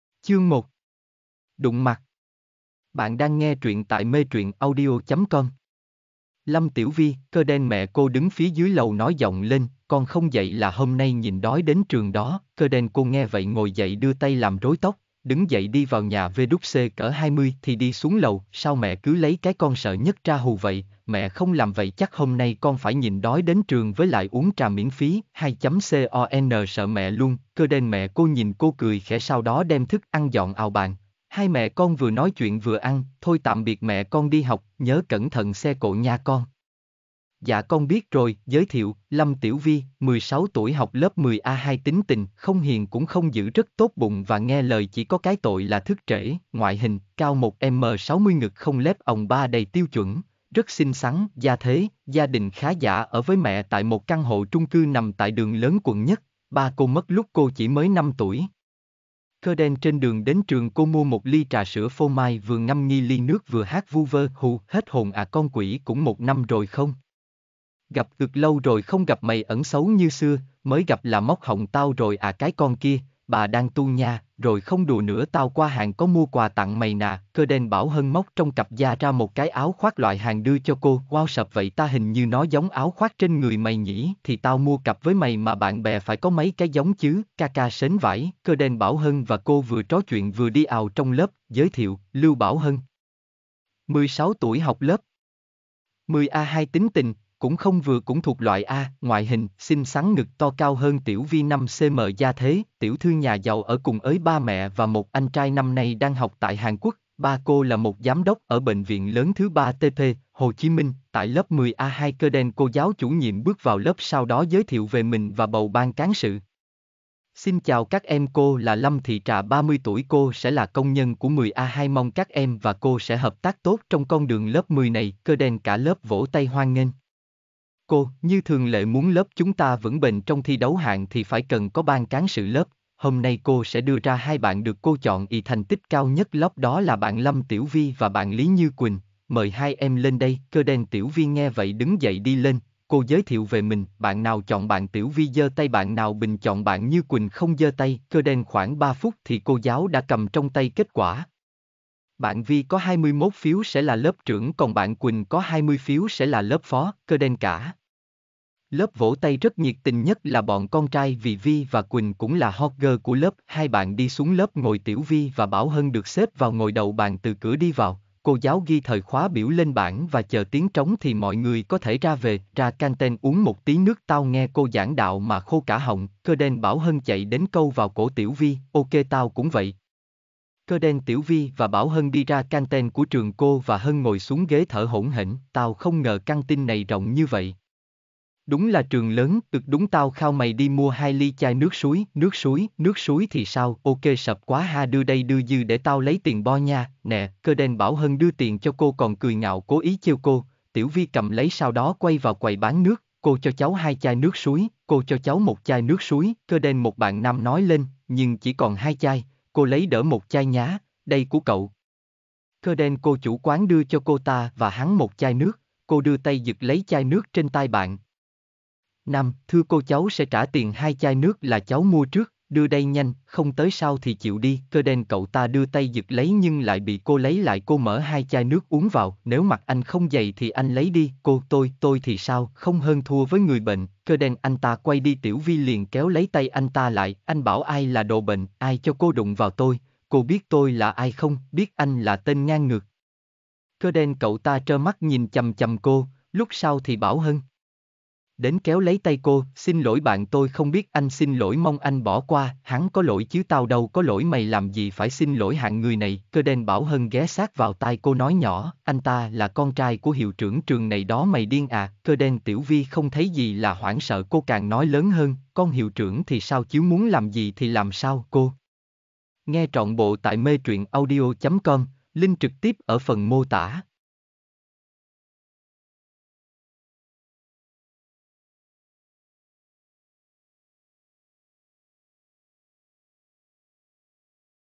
Cô Bạn Gái Cùng Trường Audio Trọn Bộ - Chương 1 | Kim Young Su Truyện Teen
Truyện: CÔ BẠN GÁI CÙNG TRƯỜNG Tập 1
Tác giả: Kim Young Su
Thể loại: Truyện Teen